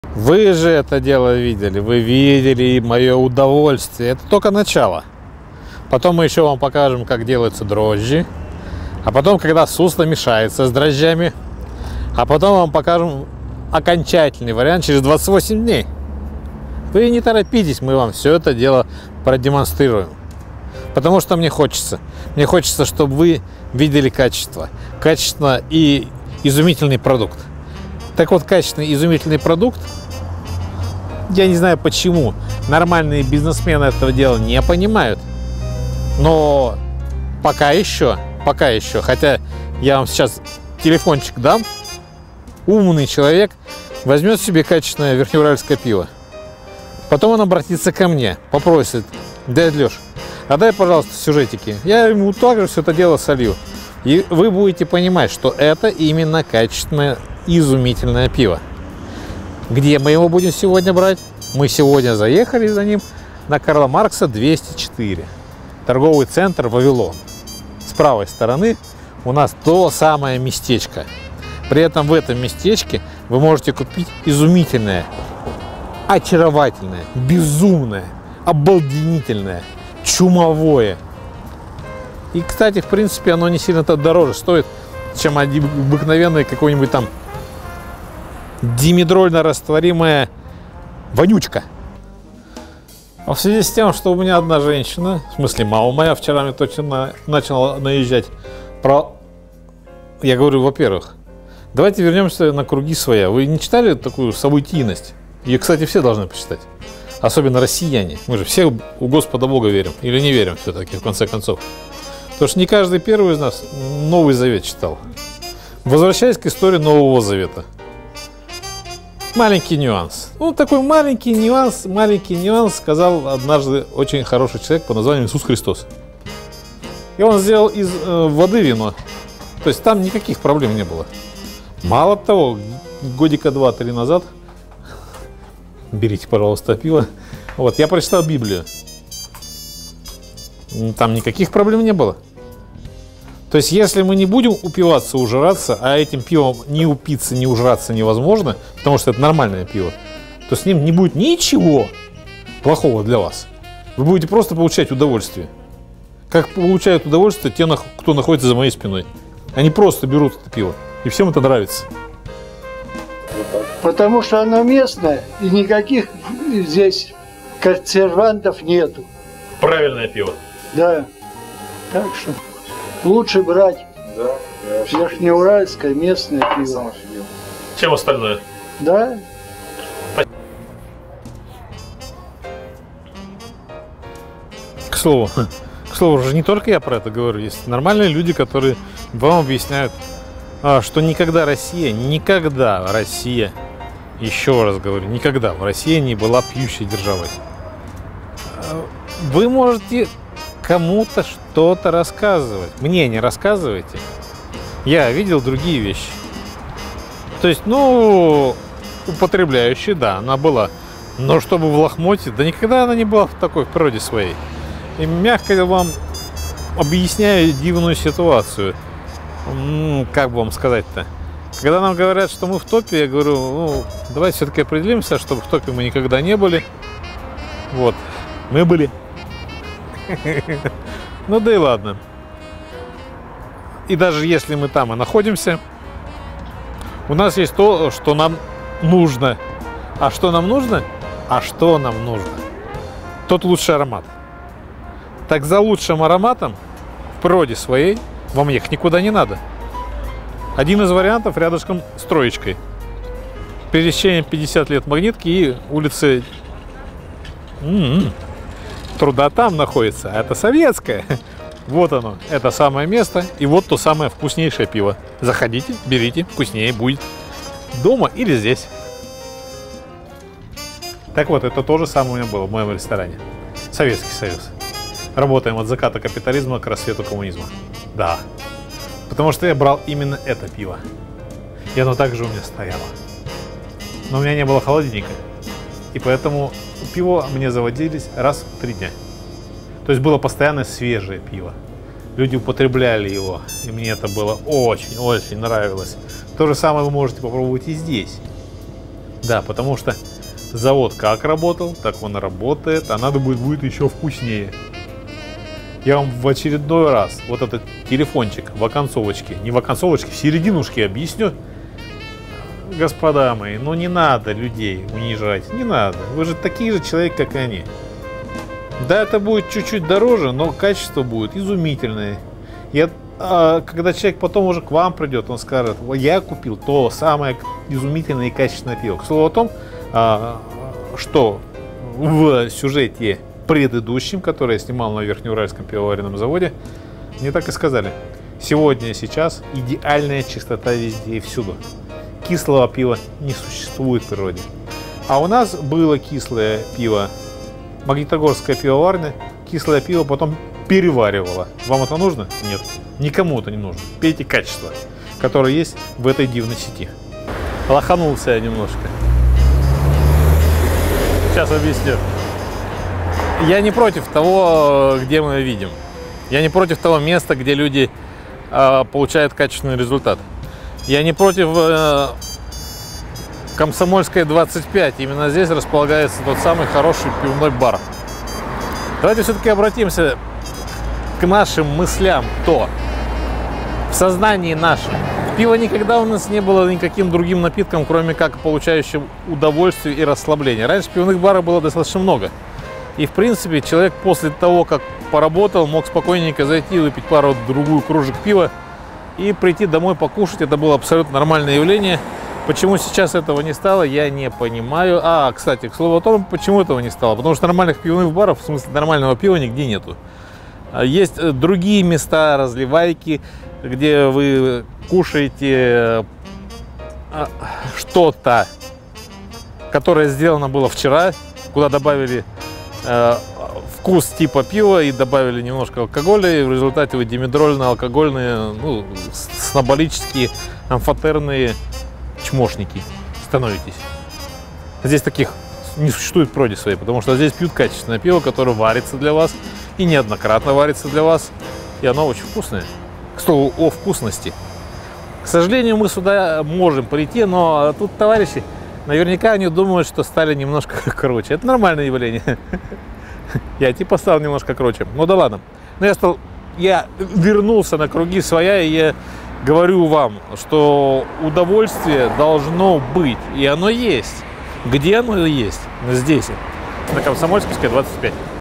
The best world beer made in Verchneuralsk 2023 9 1
Челейкин Алексей - лучшее пиво в мире сделанное в Верхнеуральске как мнение пенсионера, Cheleykin Alexey - pensioner about the best world beer made in Vechneyralsk